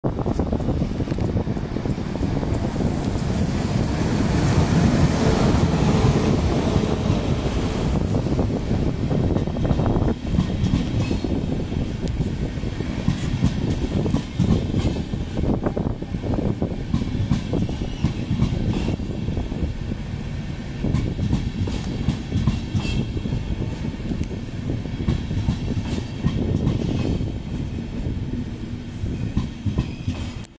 115 205 Einfahrt Hamburg - Altona
115 205 mit nem IC am Haken bei der Einfahrt in Hamburg - Altona